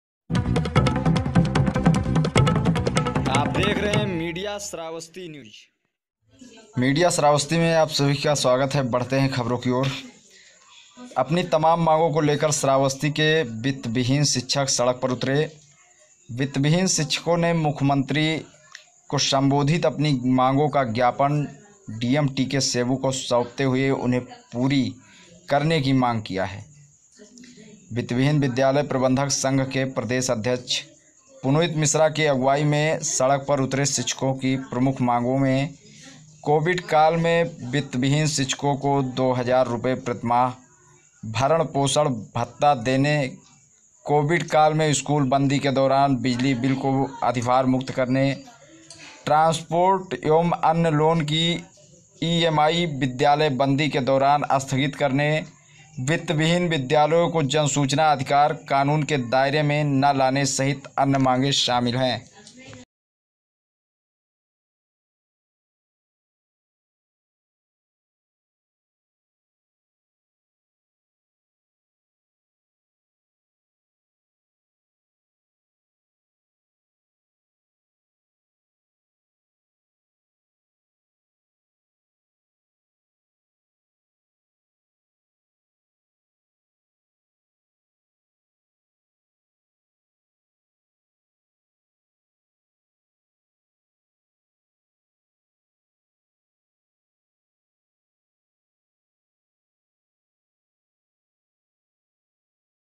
पर उतरे शिक्षक
सड़क पर उतरे वित्तविहीन शिक्षक अपनी मांगो को लेकर मुख्यमंत्री को संबोधित ज्ञापन डीएम को सौंपा #